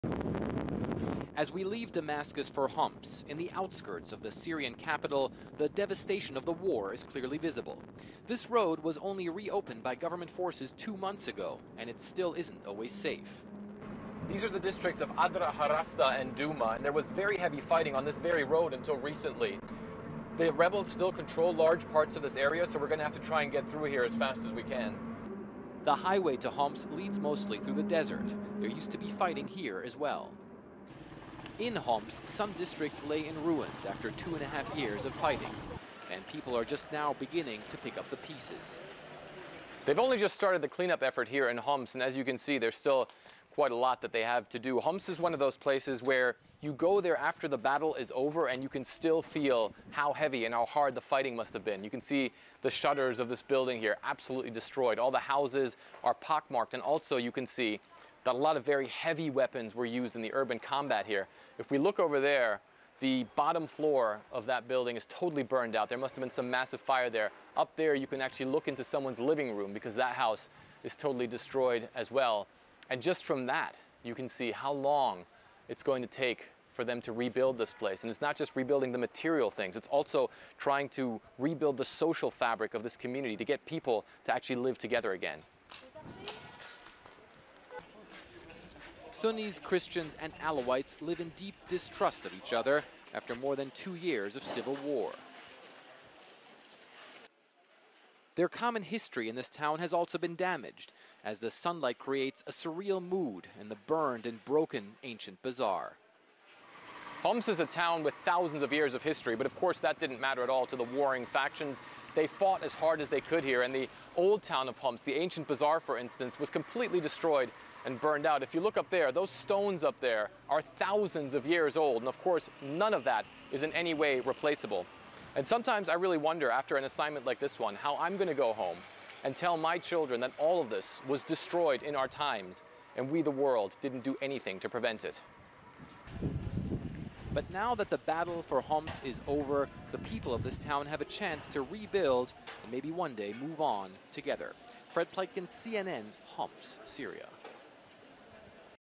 SYRIA:REFLECTIONS FROM HOMS
Homs had been under siege for two years cut off from the outside world. CNN's Fred Pleitgen reports from Homs.